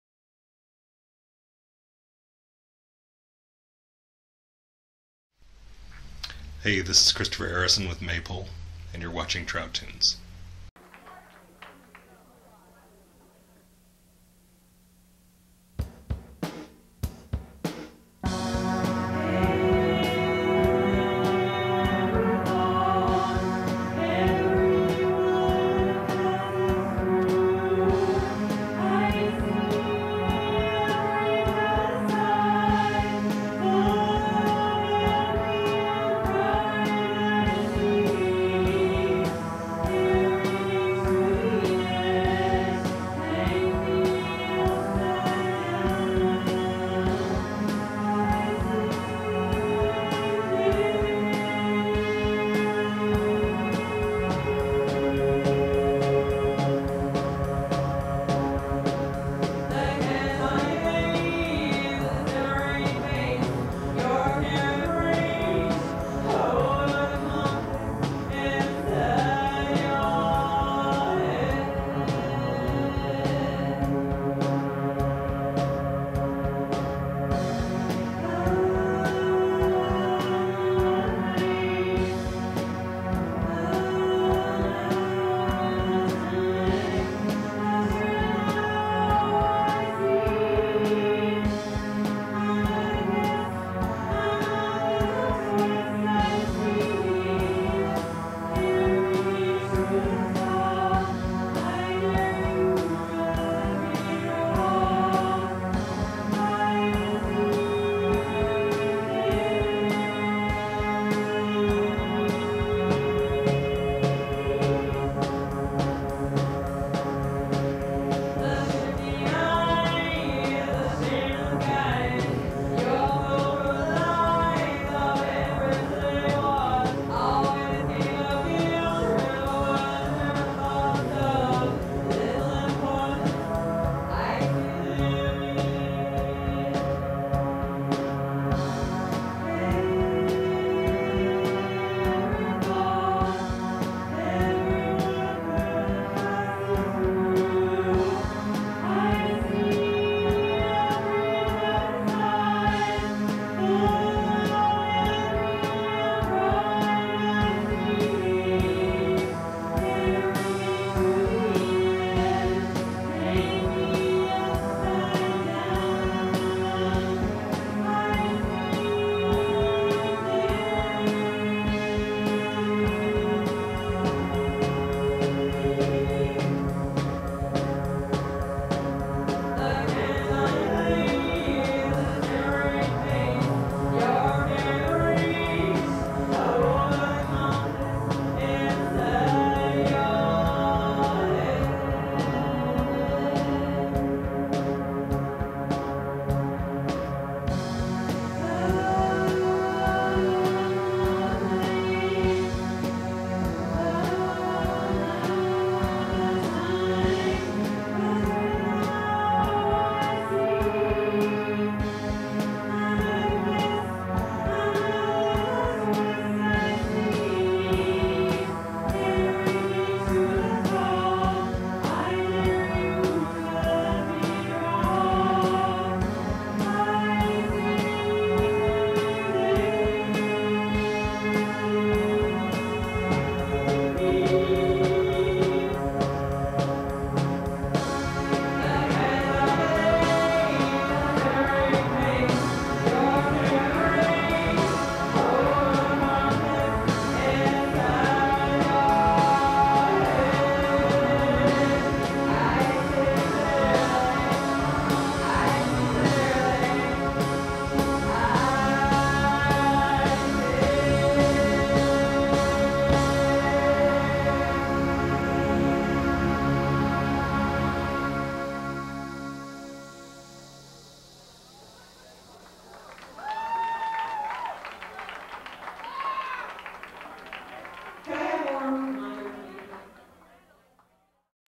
Maypole Song04 TitleUnknown HST10 12 1993
Taped at one of the last, if not the final New Music Tuesdays at the Howard St, here's Omaha's Maypole, taped for Trout Tunes October 12, 1993.  The band's line up consisted of Krystyna Eller -- Vocals, songwriter; Bart Wolfe -- Synthesizer, composer;
Christopher Erisson -- Drums and percussion.  The band was active from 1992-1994.